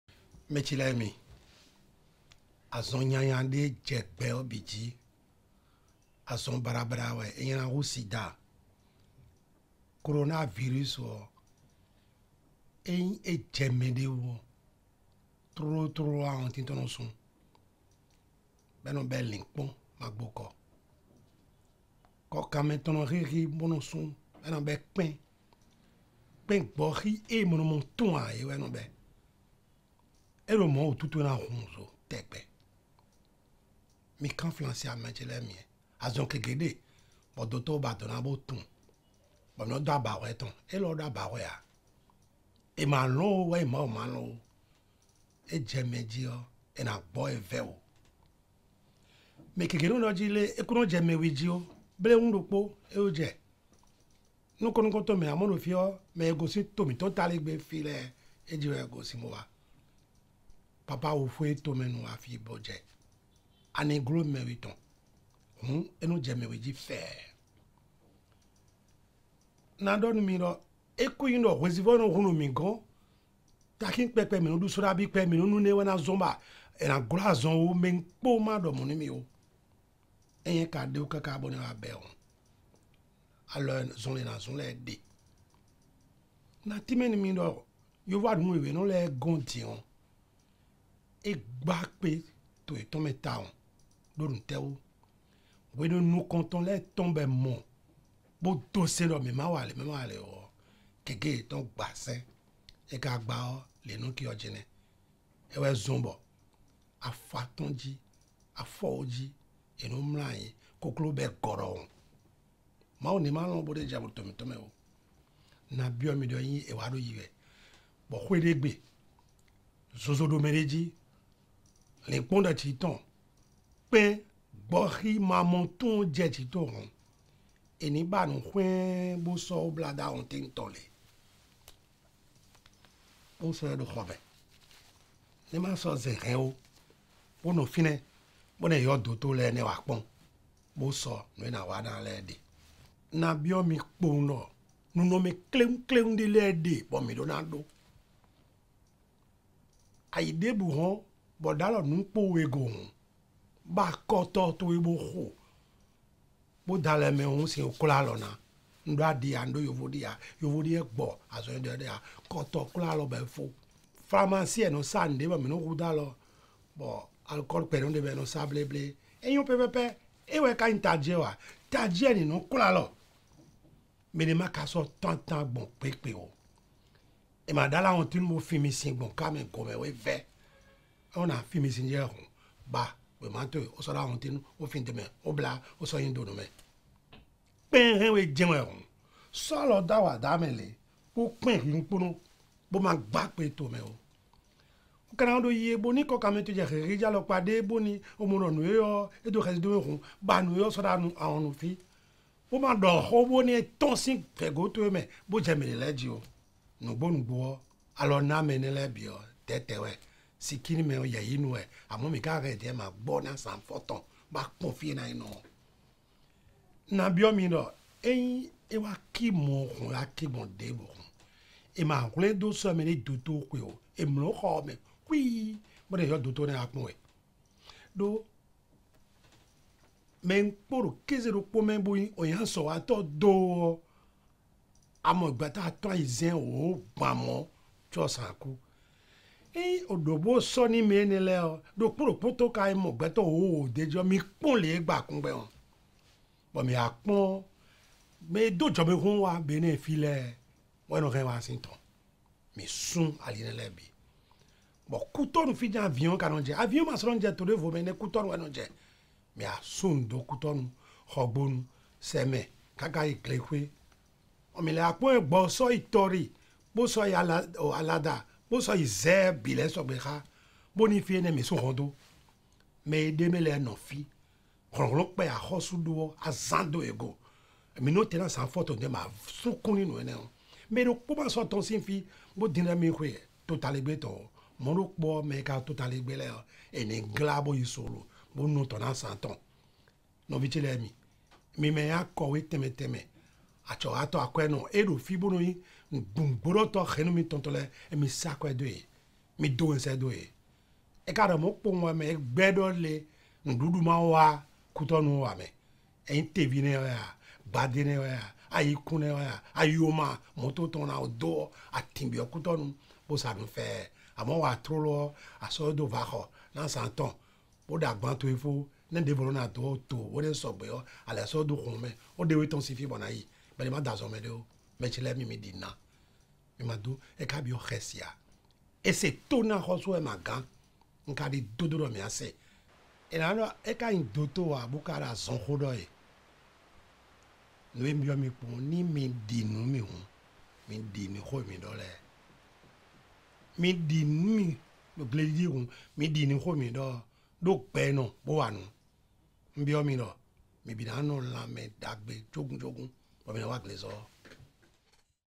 [MAHI] Gestes barrières et mesures d'hygiènes pour éviter la propagation du Covid-19 au Bénin
Message de sensibilisation du Ministre de l'Agriculture, de l'Elevage et de la Pêche (MAEP), Monsieur Gaston Cossi DOSSOUHOUI (en langue locale Mahi) au sujet des Gestes barrières et mesures d'hygiènes à adopter par tous pour éviter la propagation du Coronavirus au Bénin.